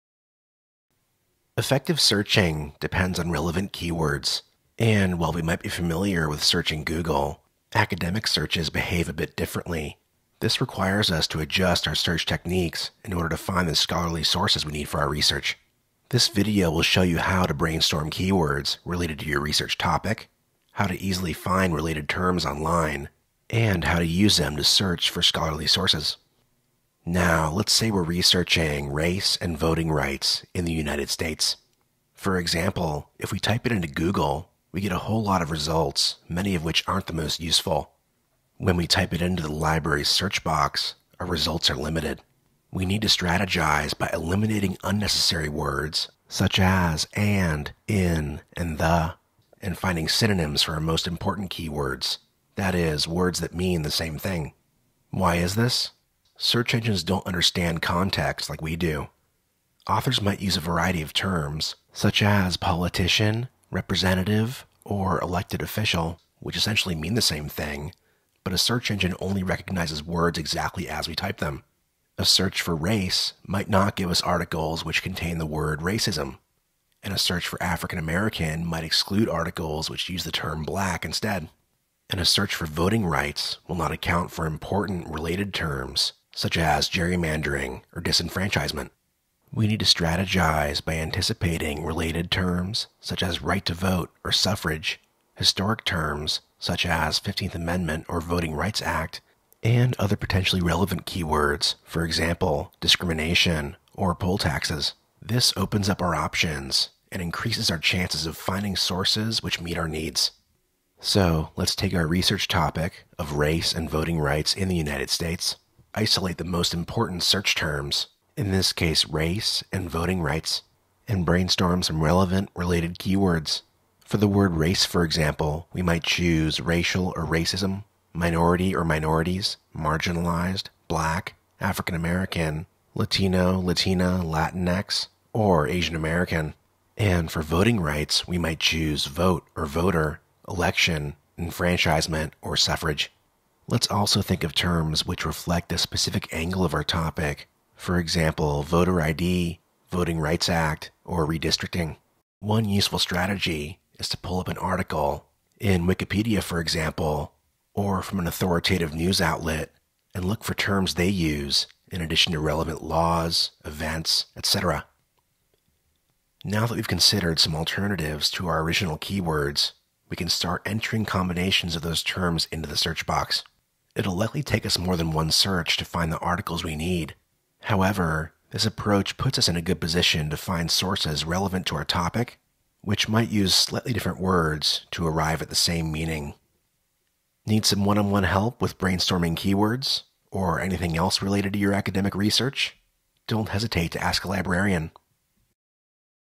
Brainstorming Keywords
Want to make your research process easier and more efficient? This video will teach you how to strategically find relevant books and peer-reviewed articles, by brainstorming and locating keywords.